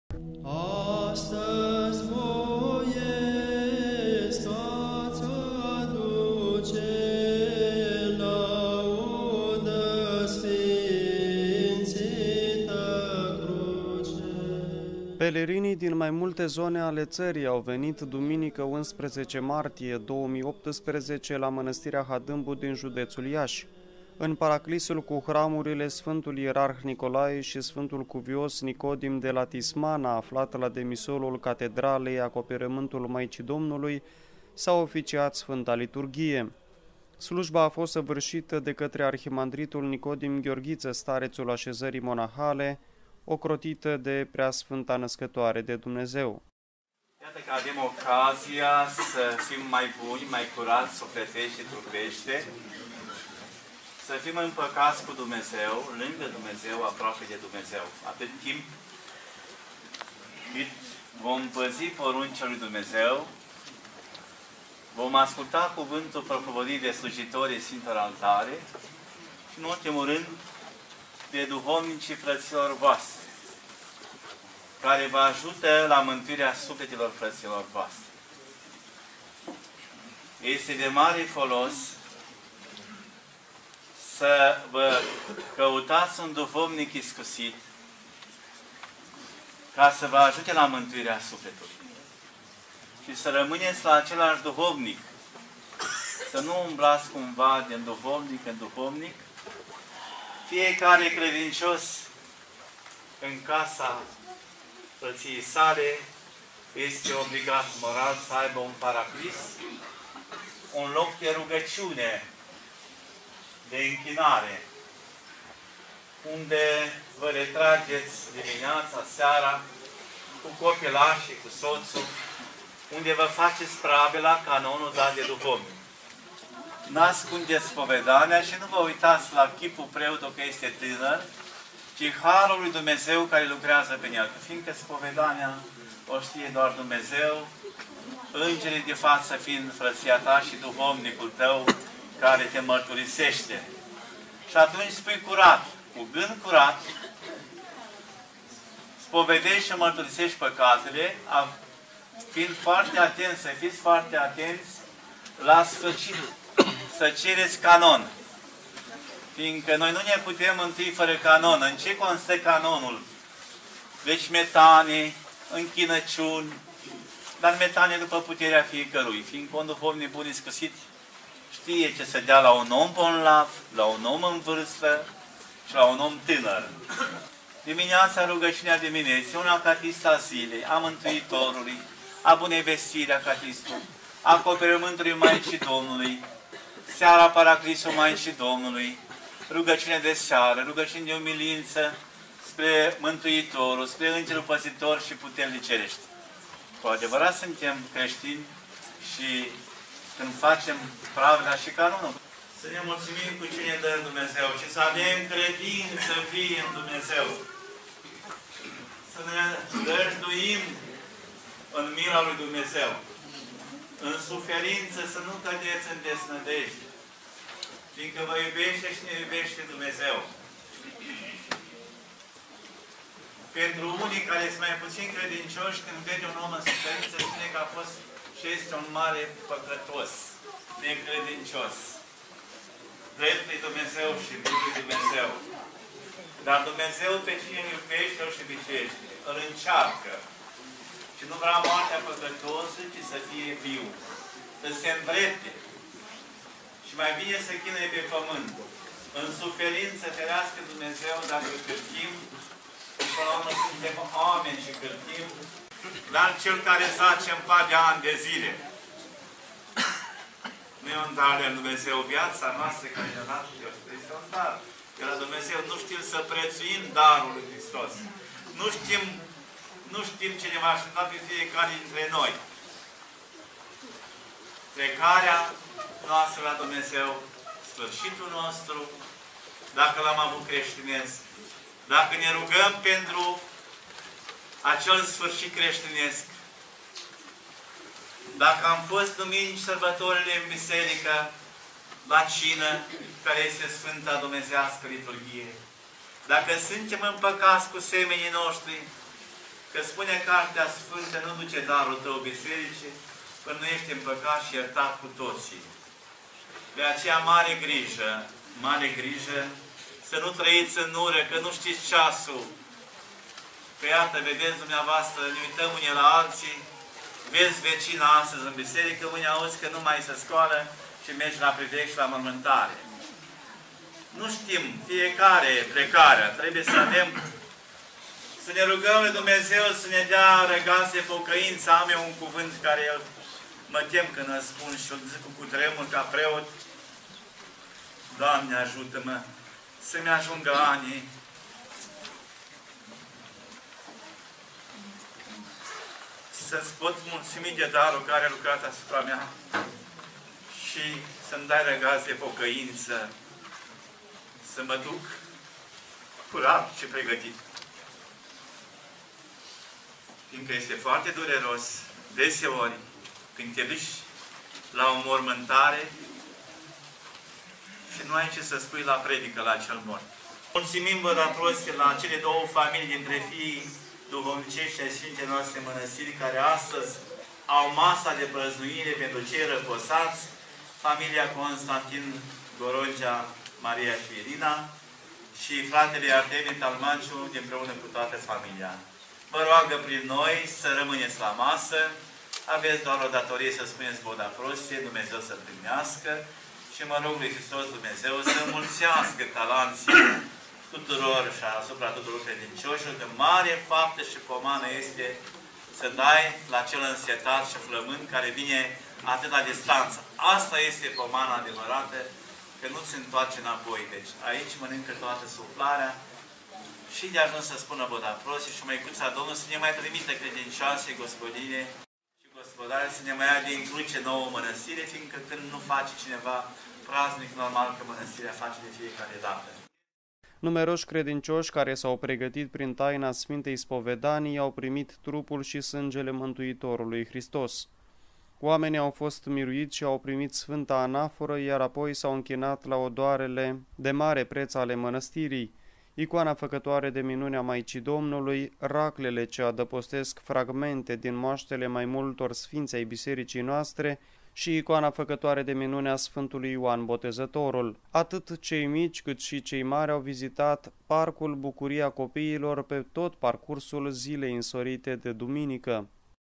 Duminica a III-a din Post la Hadâmbu - 11 martie 2018
Pelerinii din mai multe zone ale ţării au venit duminică, 11 martie 2018, la Mănăstirea Hadâmbu din judeţul Iaşi. În paraclisul cu hramurile „Sfântul Ierarh Nicolae” şi „Sfântul Cuvios Nicodim de la Tismana”, aflat la demisolul Catedralei „Acoperământul Maicii Domnului”, s-a oficiat Sfânta Liturghie.